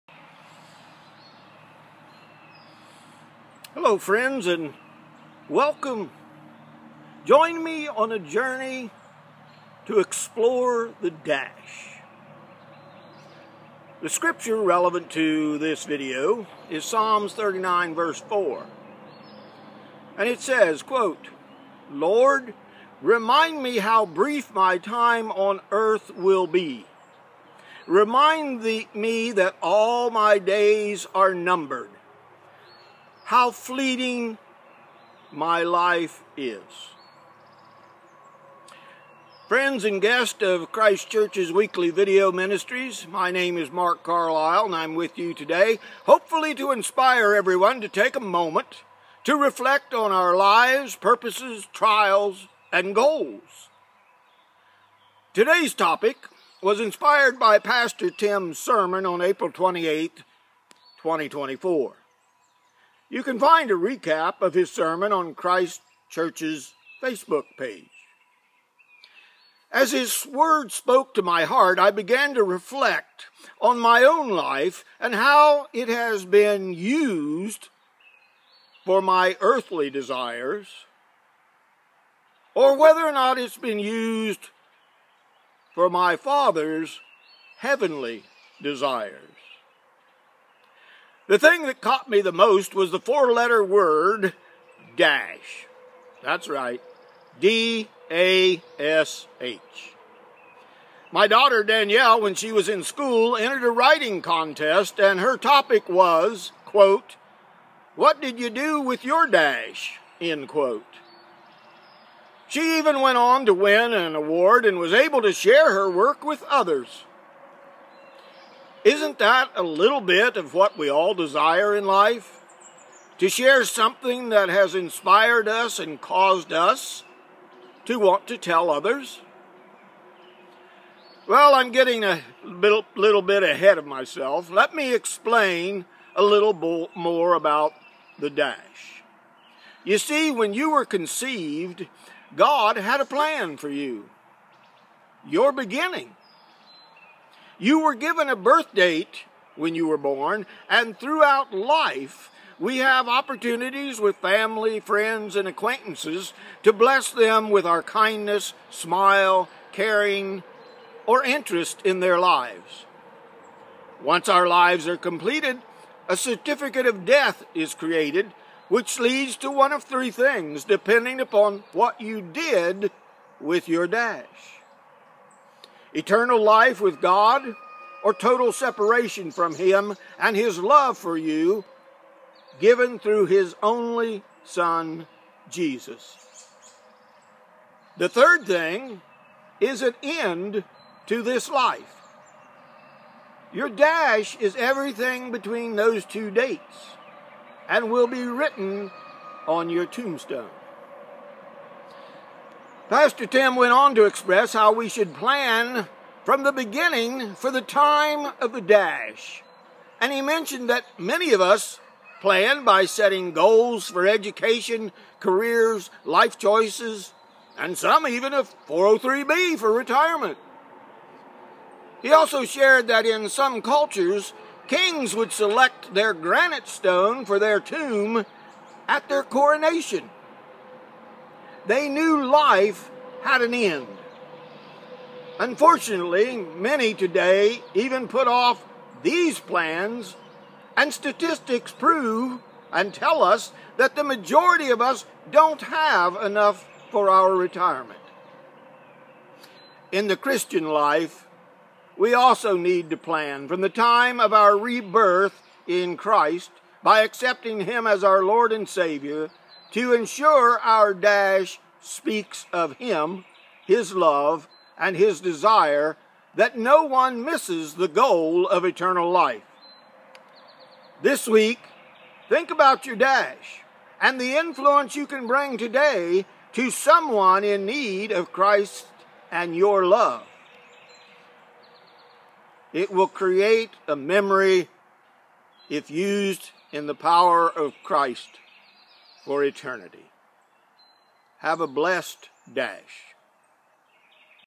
The Adventures of the DASH
Welcome friends and join us as we explore the DASH.  This video was inspired by Pastor Tim's Sermon on April 28th 2024.  You can view it on Christ Church's Facebook page.  It examines the space between our birth and death and how we use that TIME for our own earthly purpose or for our Heavenly Father's purpose for us.  He shared how we often make plans for our education, career and life choices including saving in a 403B.  But more importantly how we PLAN for the desires of our Heavenly Father and the reward of Eternal Life.